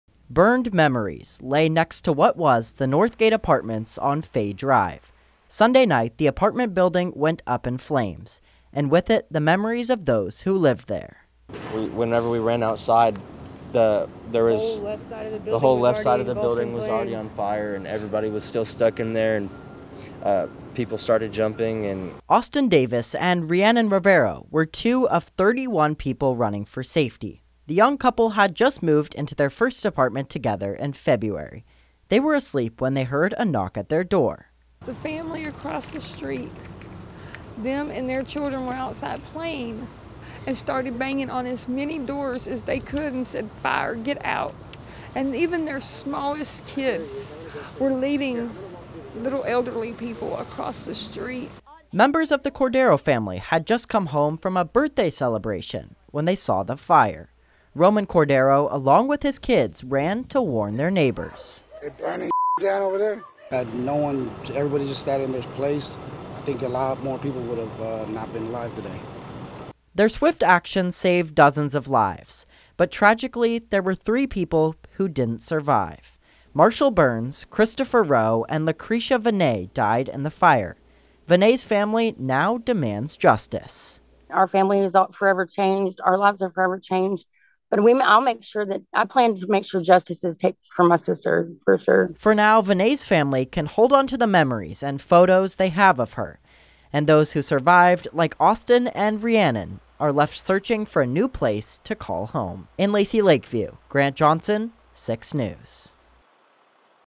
Survivors of Deadly Lacy Lakeview Apartment Fire Speak Out
Apartment fire survivors and neighbors in Lacy Lakeview detail rescue efforts. Witnesses say they saw people jumping from the two-story building and neighbors helping people get out.